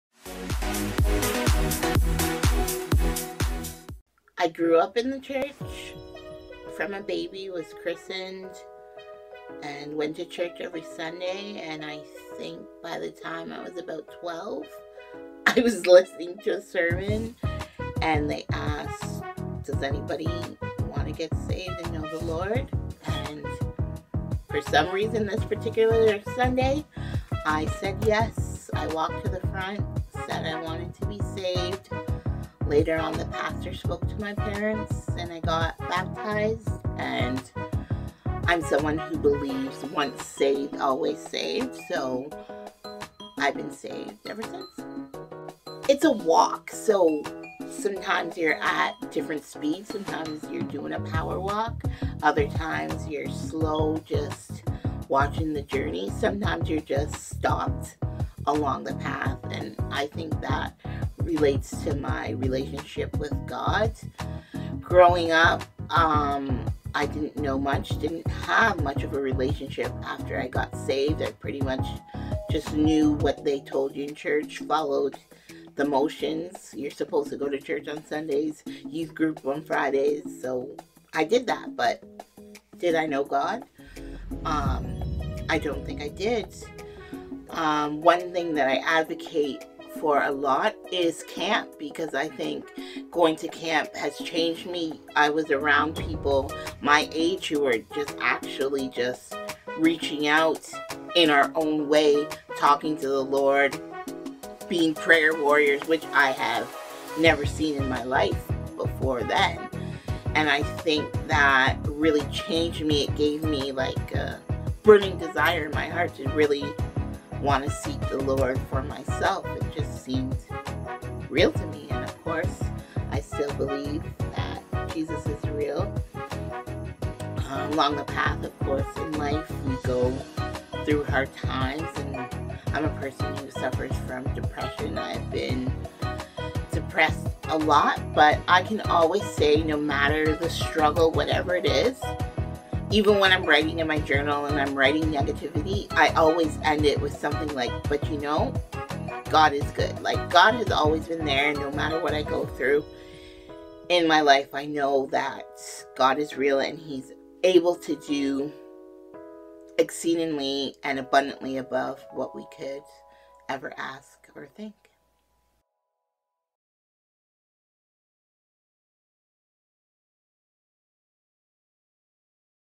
How I got saved||My walk with God
Hi everyone!
My friend Christine shares with us how she got saved and tells us a little bit about her walk with God.  She was saved at a young age, however, her relationship with the Lord started later on.  Even when life gets tough, she has learned to give God thanks and praise at all times.